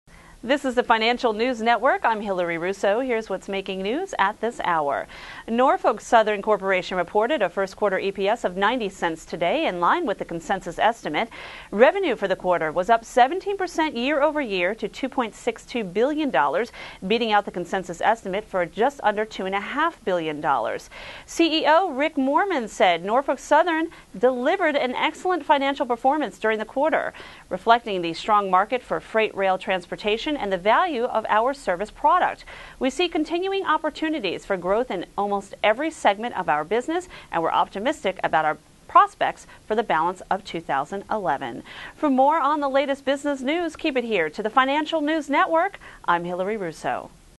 Norfolk Southern Posts Solid Q1, EPS In-Line, Revs Beat Up 17% YoY
Norfolk Southern Corporation (NYSE:NSC) reported Q1 EPS of $0.90 today, in-line with the consensus estimate. Revenue for the quarter was up 17% year-over-year to $2.62 billion, beating the consensus estimate for $2.48 billion.

CEO Wick Moorman said, "Norfolk Southern delivered an excellent financial performance during the quarter, reflecting the strong market for freight rail transportation and the value of our service product. We see continuing opportunities for growth in almost every segment of our business, and we're optimistic about our prospects for the balance of 2011."